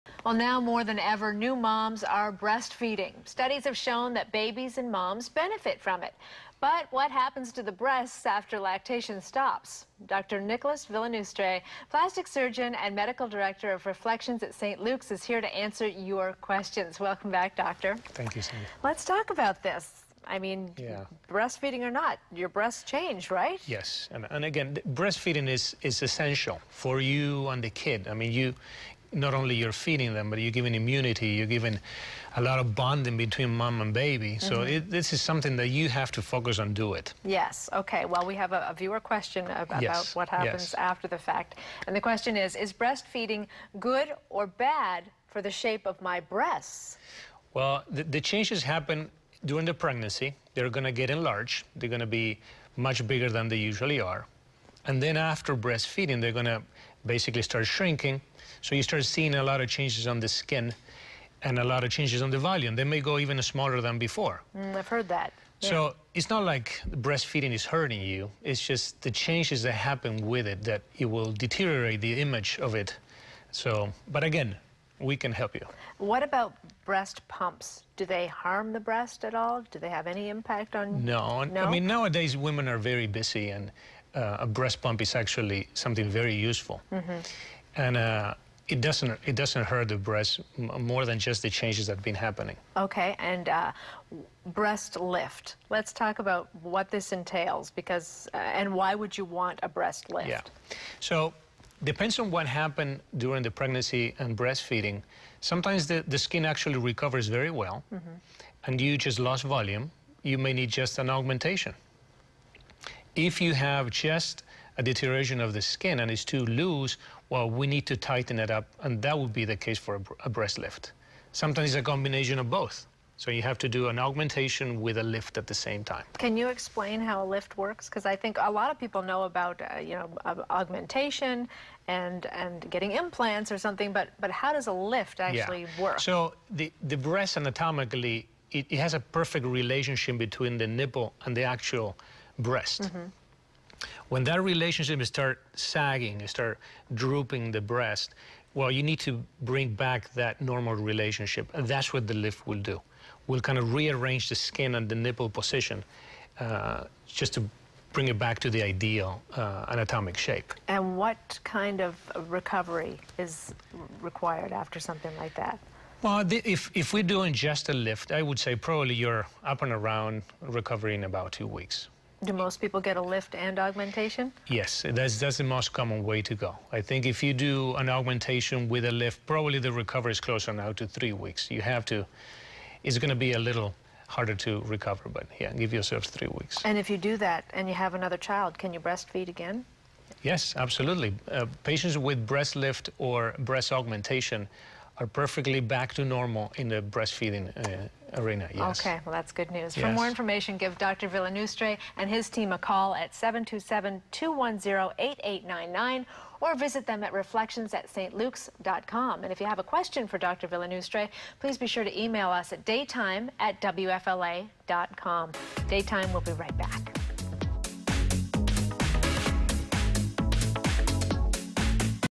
Dr. Nicolas Villanustre - Reflections at St. Luke's: The Effects of Breast Feeding and Lactation
Dr. Nicolas Villanustre - discusses The Effects of Breast Feeding and Lactation